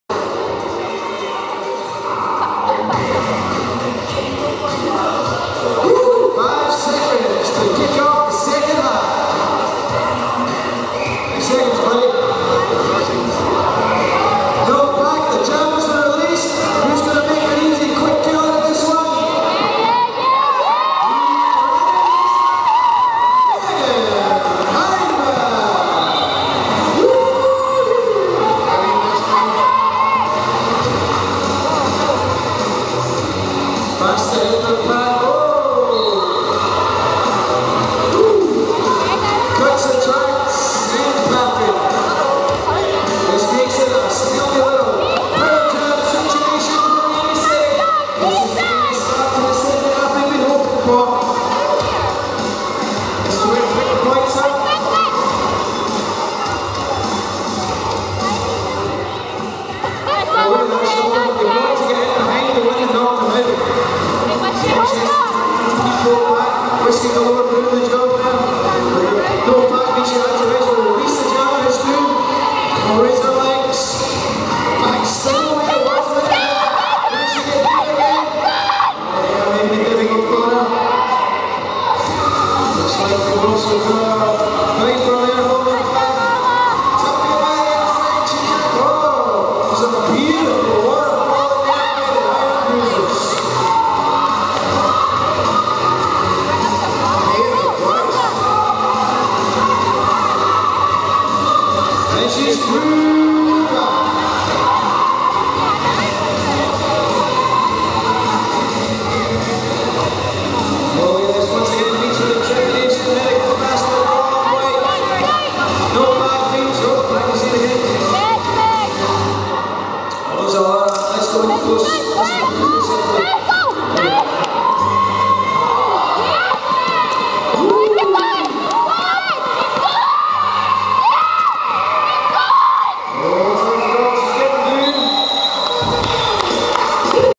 GRG Irn-Bruisers vs Rainy City Roller Girls 29 Oct 11 Jam 1 Period 2
GRG Irn-Bruisers vs Rainy City Roller Girls
29 Oct 11
The ARC, Glasgow Caledonian University

This is the 1st Jam of the 2nd Period. Your jammers are Megan Hyndman for GRG and Randi Razorlegs for RCRG. It becomes a power jam for RCRG.

Your announcers were Handsome Joe Hanson and Helliverance